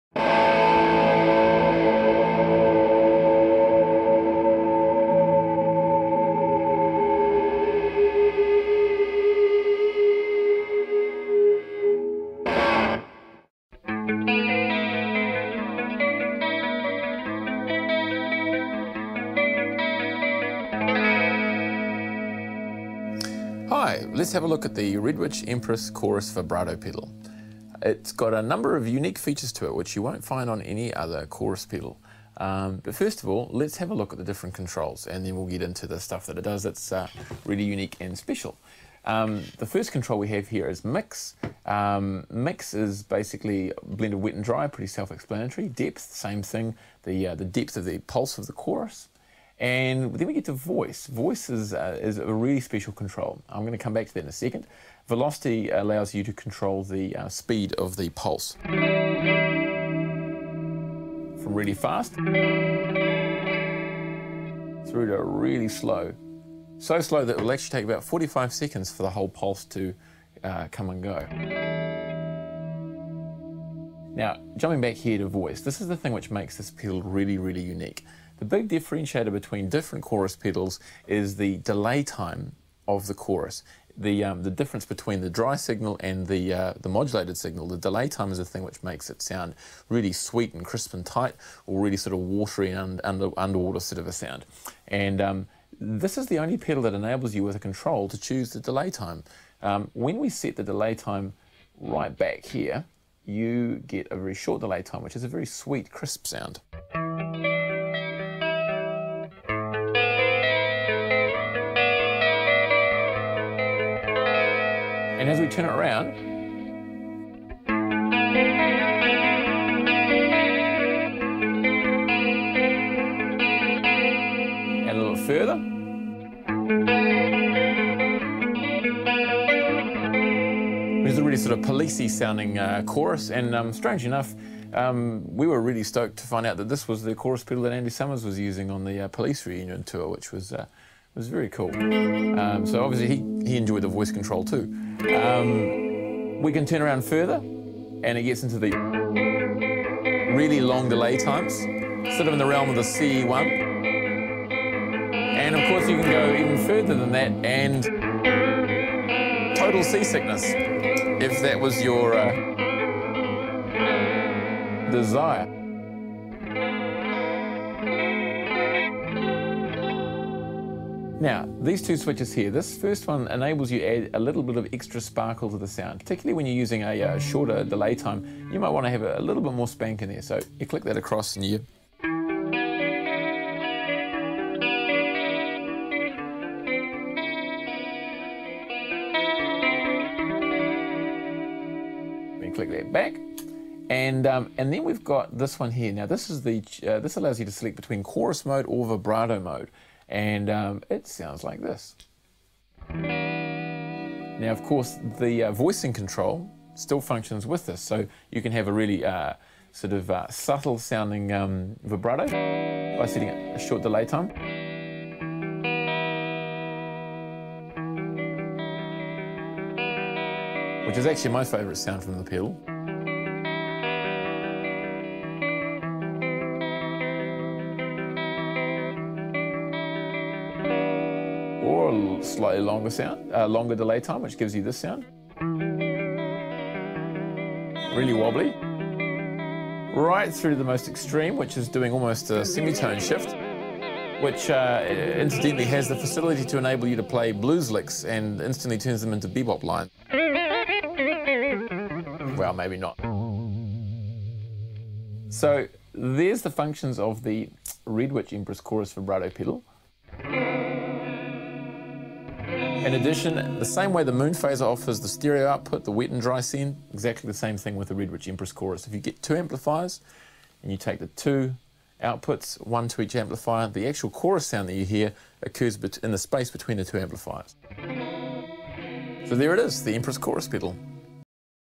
Red Witch Empress Chorus Vibrato Pedal Video Demo
Not just any ordinary analog Chorus/Vibe pedal.

Born of the same stock as the Red Witch Medusa chorus-trem , the Empress offers
unparalleled chorus options.

An infinite number of analog chorus/vibe devices in one box...

Her most enticing feature is the unique voicing control; This dial allows you to access virtually any chorus sound you could desire! It changes the delay time  its on a pot so youve got an infinite number of chorus pedals in one box  from super spanky sparkling clean to noisy, sea sick inducing, pitch bending wobble.